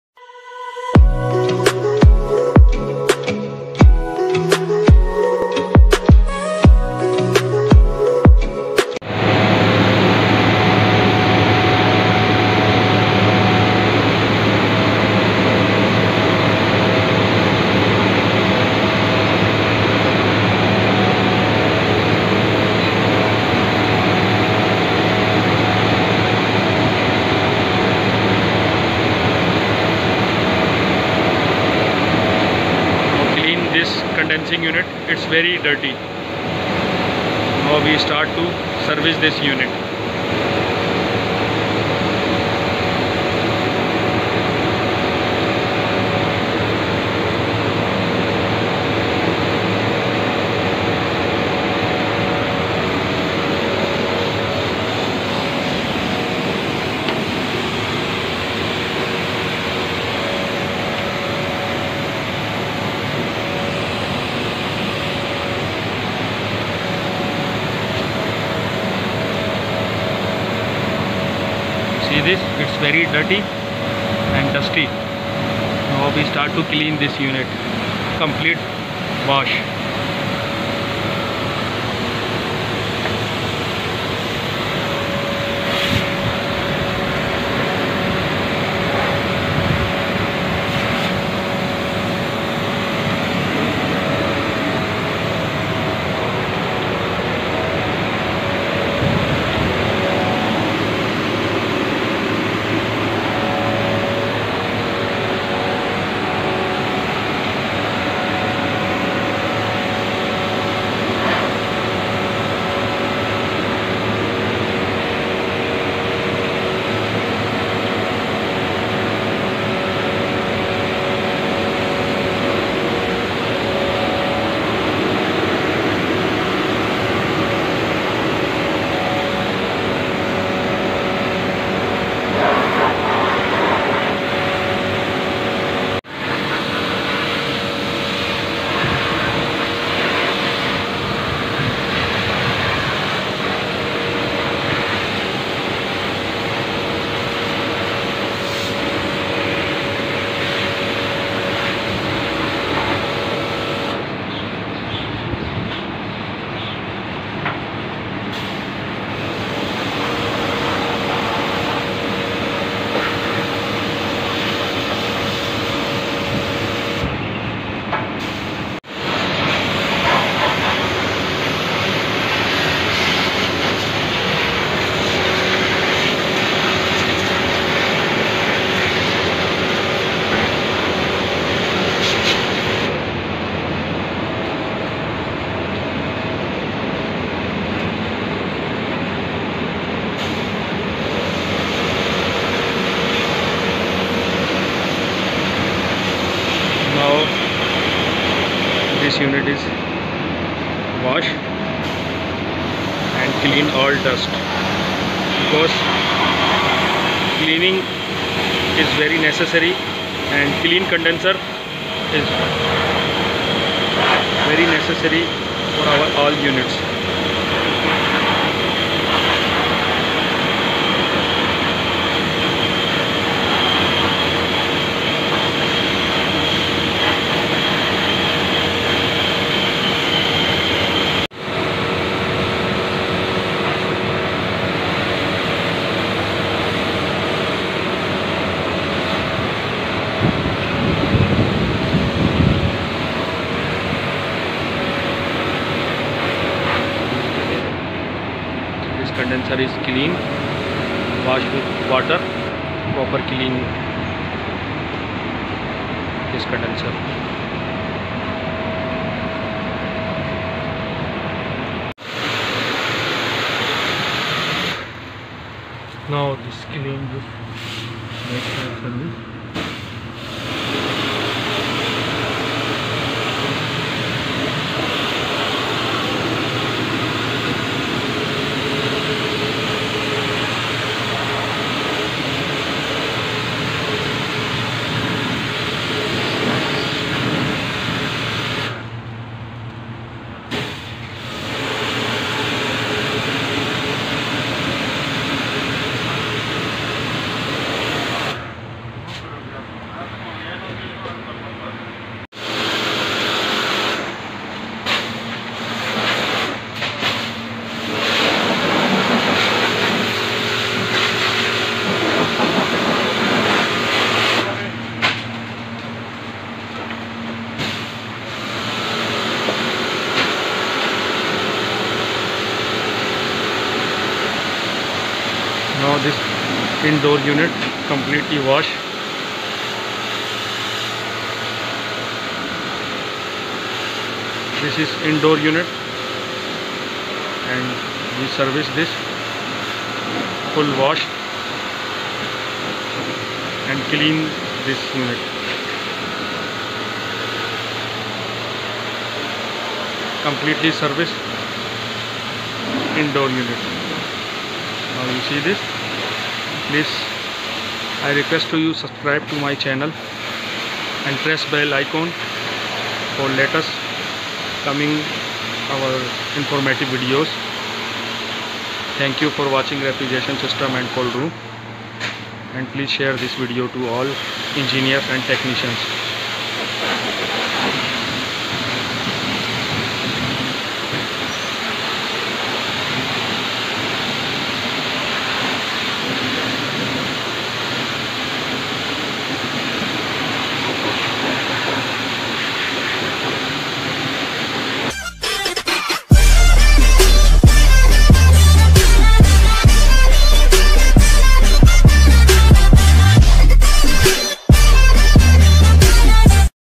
How To Clean Condensers || Refrigeration System
This Video We Showing How To Clean The Refrigeration System (Condenser And Evaporator)

The Benefits Of Clean The Condensers And Evaporators Coils Is Energy Savings, Maximum Heat Transfer, System Longevity And Improved Air Quality.